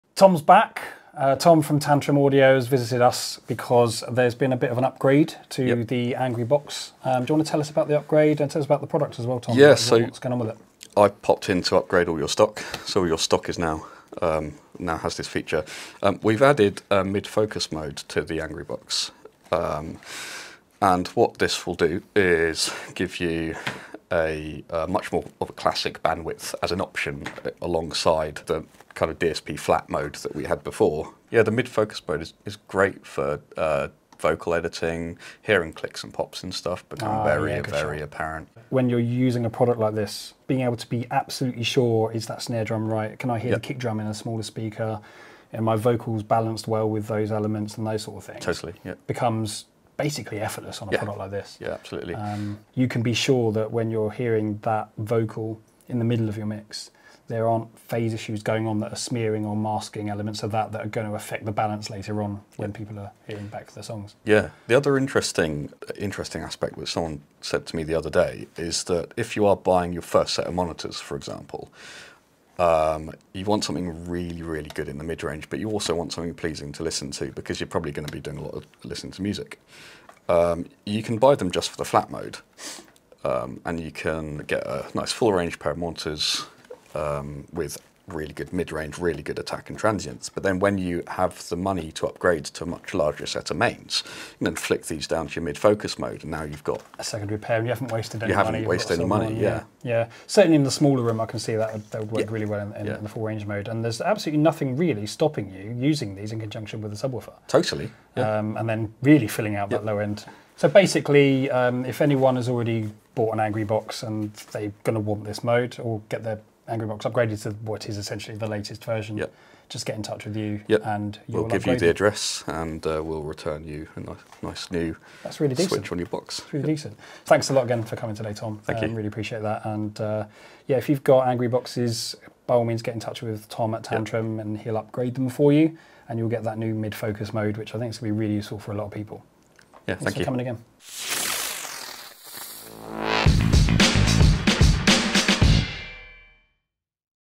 The Angry Box just got twice as Angry! Tantrum Audio Angry Box gets big upgrade!
Tom of @TantrumAudio has visited us recently to perform an upgrade to all our stock, bringing it to the latest revision which includes the much-requested "mid-focus" mode switch, allowing for a more "traditional" mix-cube-type sound. We sat down with Tom to discuss this and more!